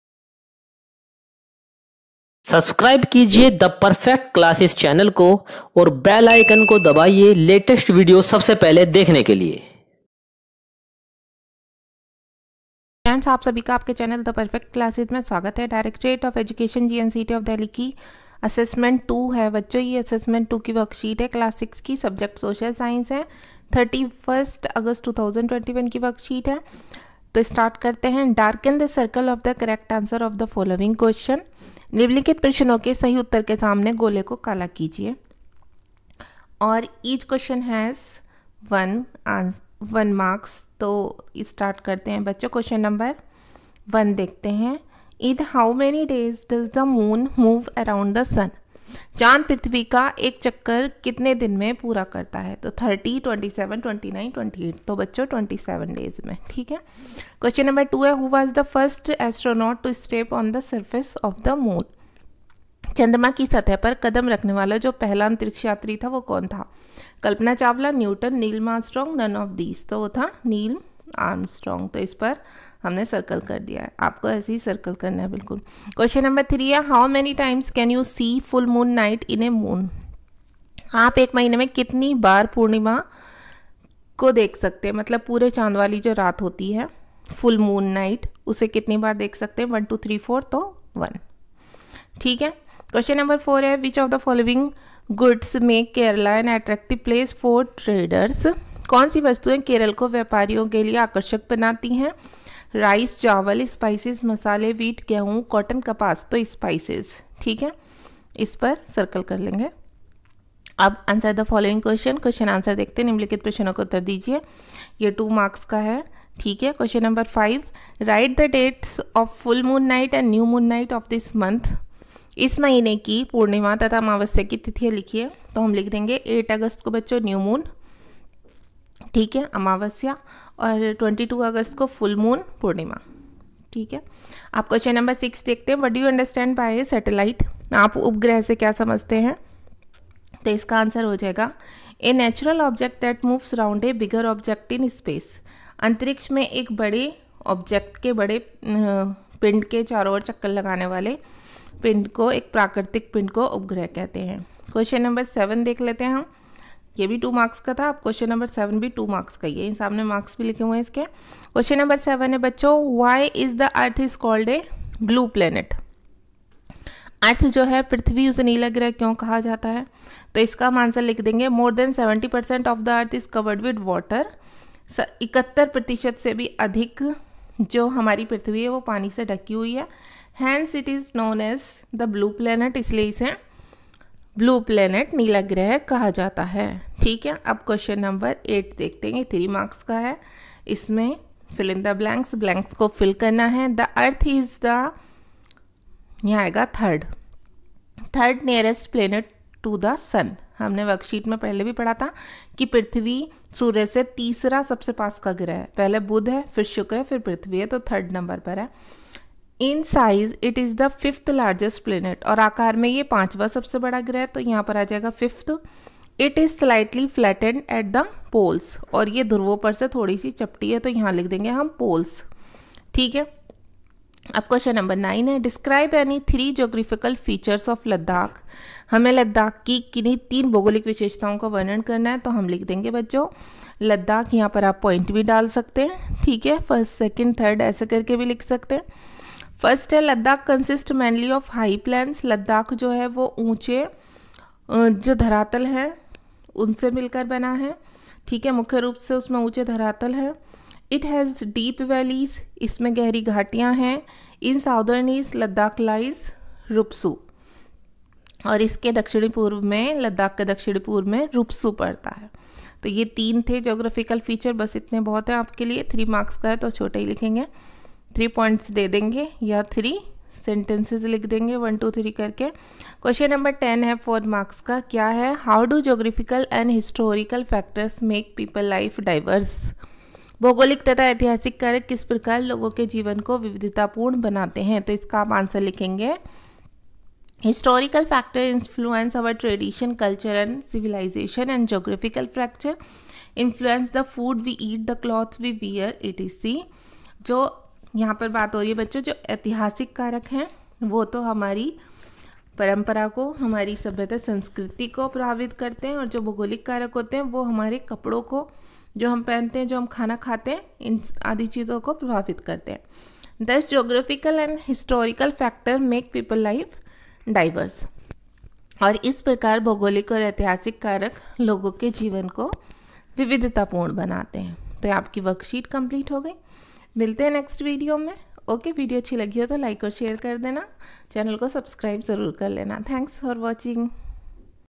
ASSESMENT 2 CLASS 6 SST (ENGLISH MEDIUM) TUESDAY 31 AUGUST 2021||(31/08/2021)
maine is video ke madhiyam se DOE, DELHI  WORKSHEET 1  SUBJECT SOCIAL SCIENCE (ENGLISH MEDIUM)  FOR CLASS 6   AUGUST   2021 KO SAMJHAYA HAI..